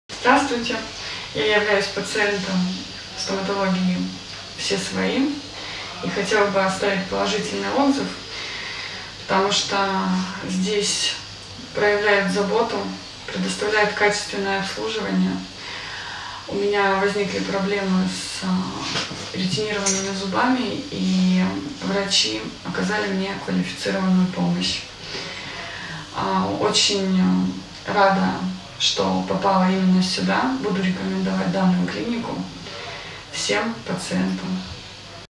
Отзыв о клинике «Все свои!» на Алексеевской
Отзыв пациентки о клинике на Алексеевской.
Приглашаем и вас на бесплатную консультацию!